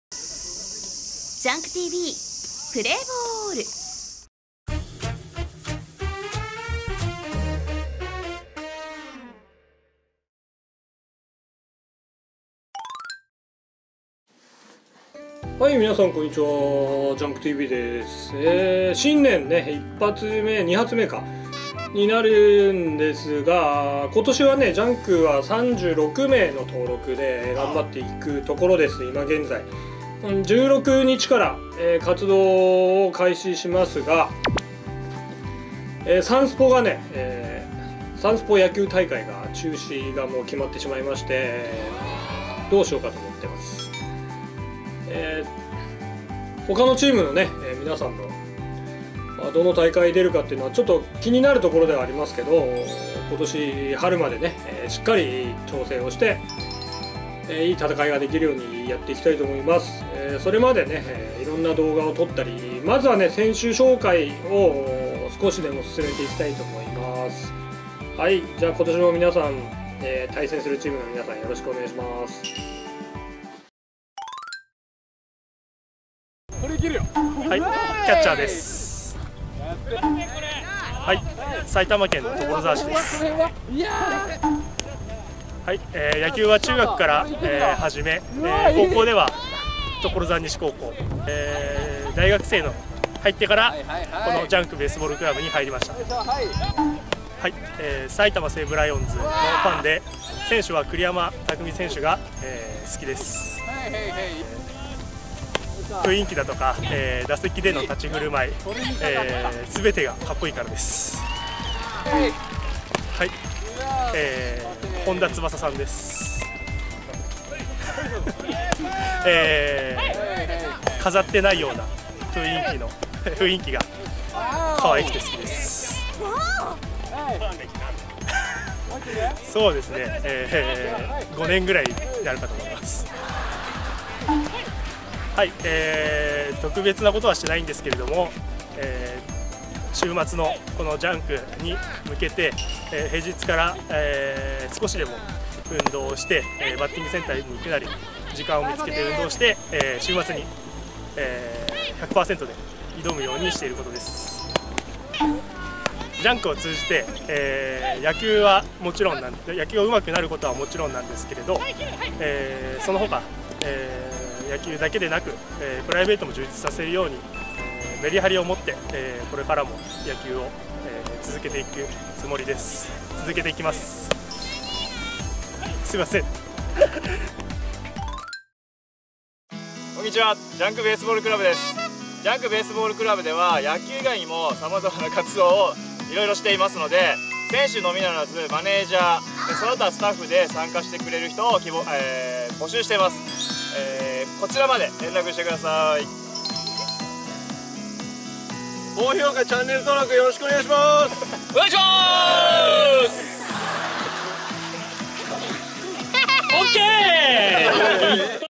2021年選手紹介また再開！第7弾！個性派軟式野球チームにふさわしいある陸上アスリートそっくりさんが登場！！
2021年最初の選手紹介の動画配信です！陸上界のスーパーアスリートそっくりさん登場！キャラも最高なメンバーです。ジャンク在籍も長く、副キャプテン！そんなおもしろキャラに熱い声援お願いします！！